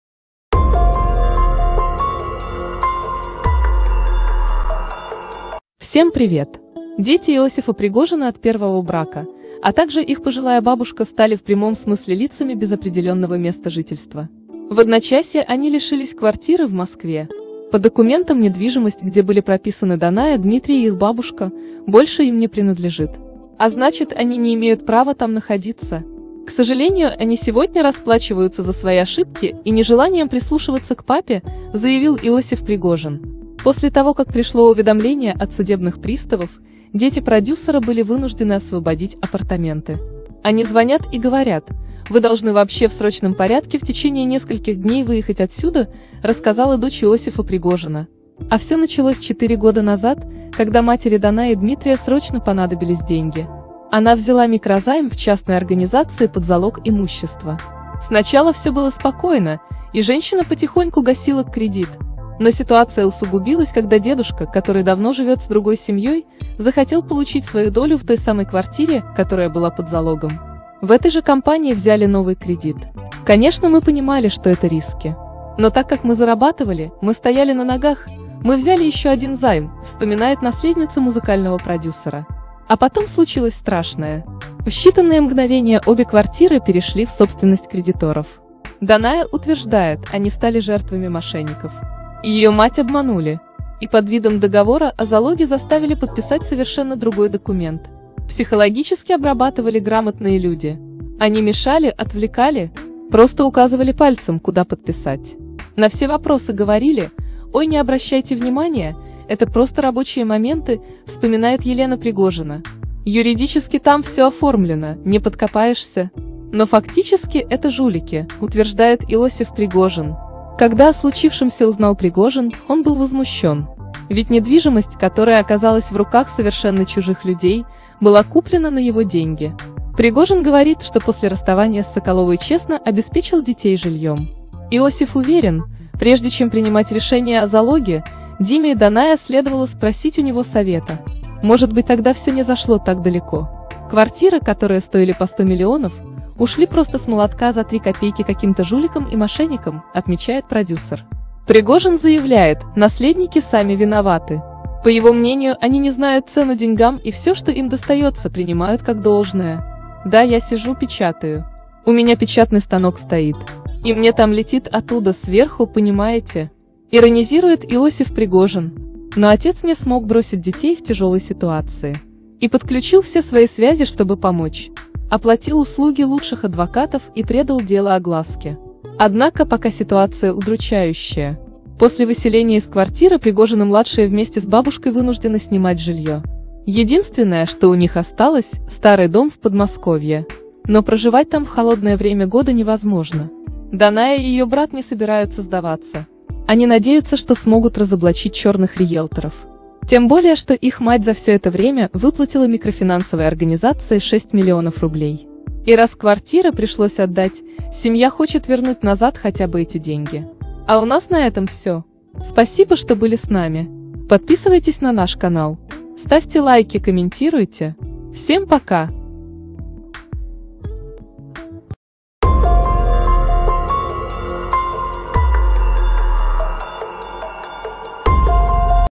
Расплата за ошибки! Дети Пригожина стали бомжами.
Каким образом дети Пригожина стали бомжами? Продюсер пришел в ярость от безрассудного поведения наследников.